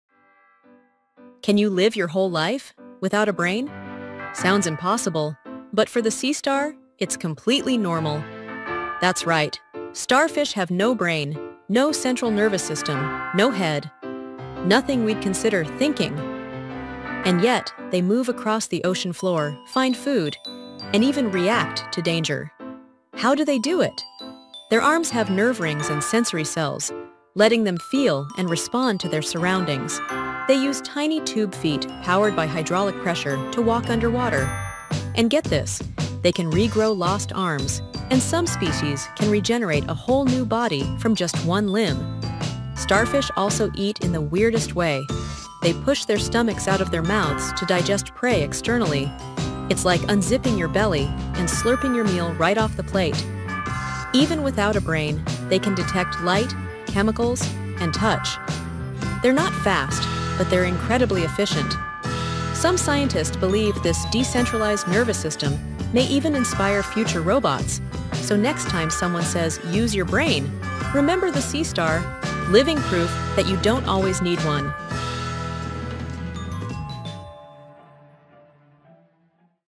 The Animal That Can Live Without a Brain!
Think you need a brain to survive? Not always. Meet the sea star—yes, the starfish. This bizarre marine creature can move, hunt, and even digest prey… all without a brain. Dive into the strange world of animals that challenge what it means to be “alive.”